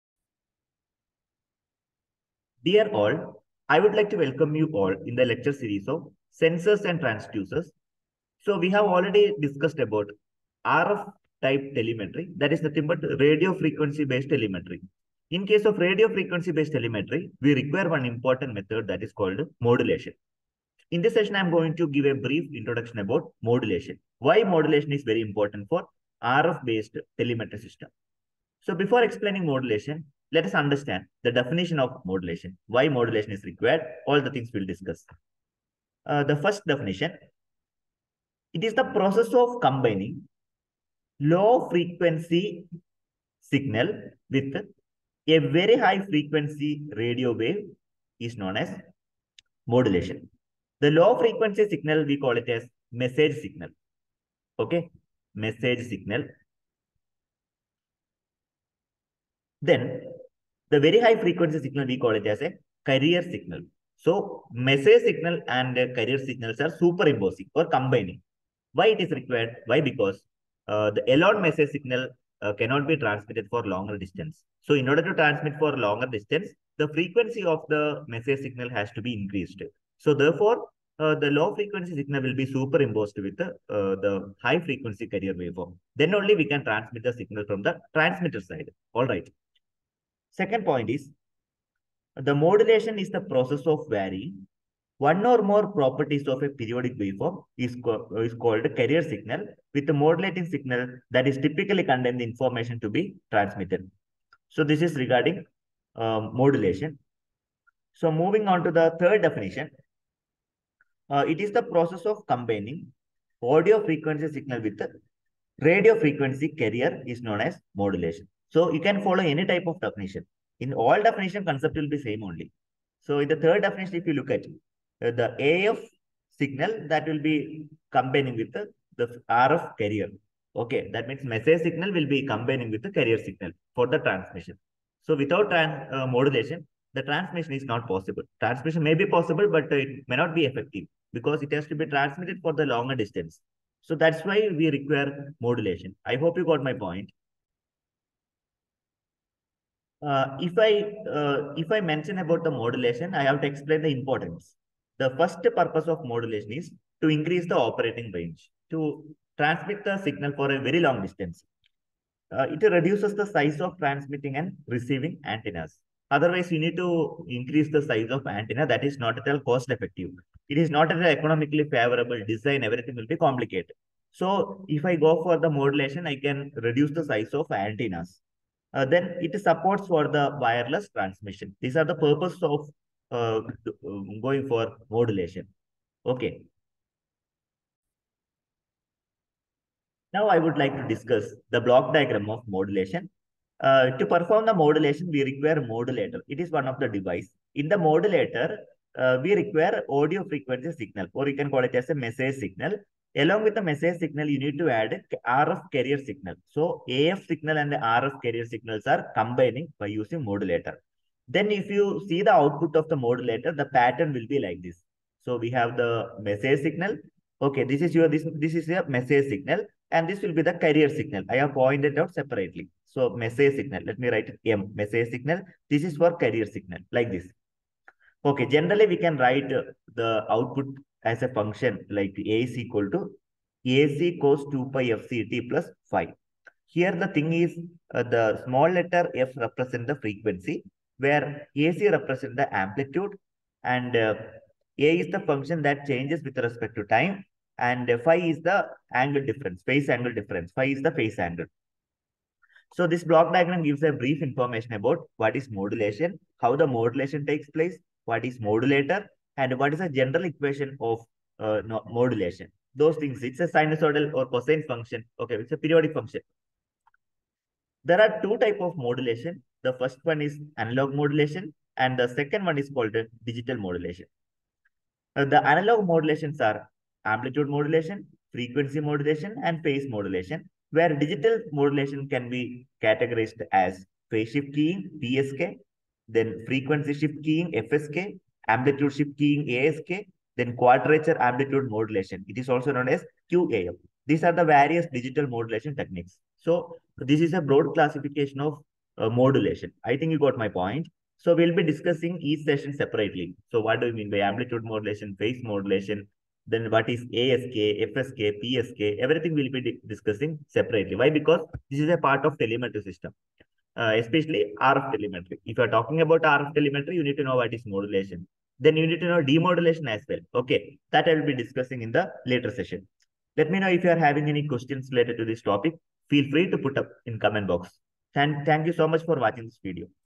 What is modulation? Need for Modulation|Modulation in Radio Frequency (RF) Telemetry system|S&T VTU
Dear all,
In this video, I've explained the need for modulation in RF telemetry.

I will come up with similar kinds of classes in the upcoming session.

Thanks for listening to this video.